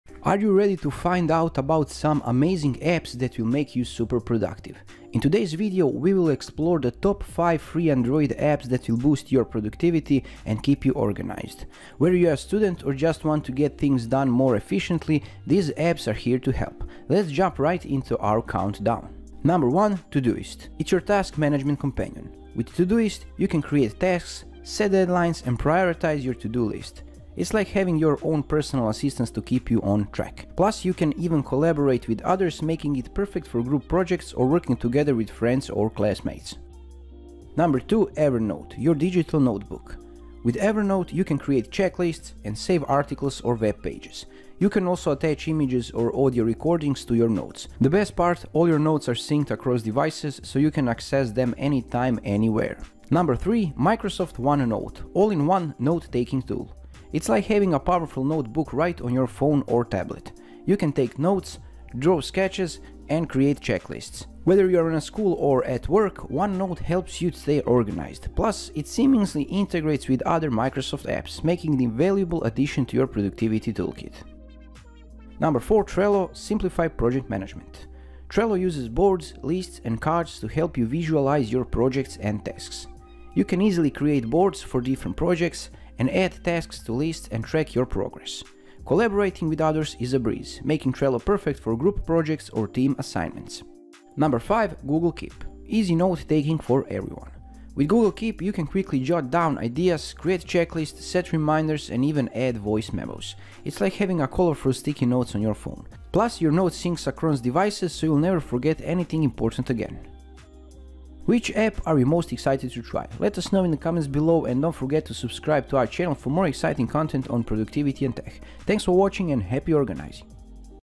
Boost Your Productivity: Discover the Top 5 Free Android Apps for Organization
In this video, we will unveil the top 5 free Android apps that will boost your productivity and help you stay organized. Whether you're a student or professional, these apps are essential tools for maximizing efficiency. Discover how these free Android apps can transform the way you work and enhance your organization skills. Don't miss out on this opportunity to supercharge your productivity!